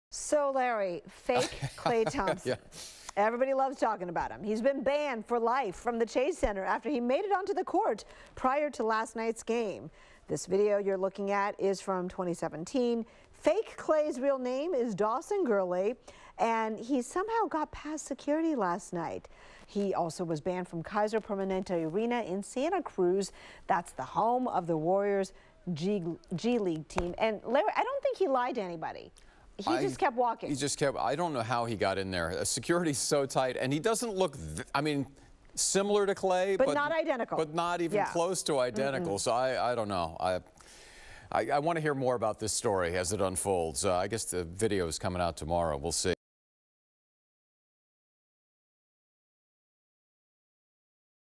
'Fake Klay Thompson' Dawson Gurley banned from Chase Center after NBA Finals stunt
"Fake Klay Thompson" has been banned for life from the Chase Center after he made it on to the court prior to Game 5 of the NBA Finals.

Fake Klay's real name is Dawson Gurley, a YouTuber, and he somehow got past security although he was wearing a full-on jersey- something athletes generally don't wear when entering arenas.